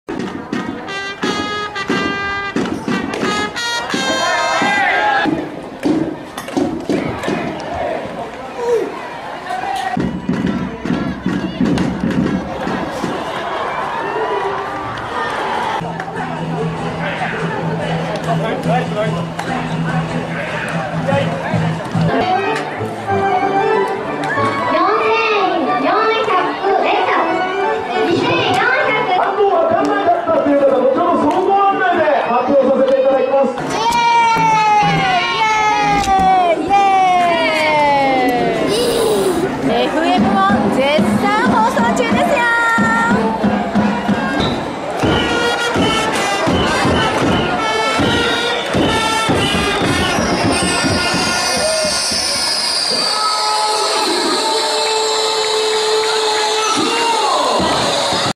Alessandro Maestri nelle indipendent league giapponesi - Kagawa Olive Guyners
Uno spezzone della Guyners Tv. Alessandro Maestri lancia per i Kagawa Olive Guyners, squadra di indipendent league giapponese.

I Guyners fanno parte della lega Shikoku Island plus.
Un video globale con Alessandro che lancia.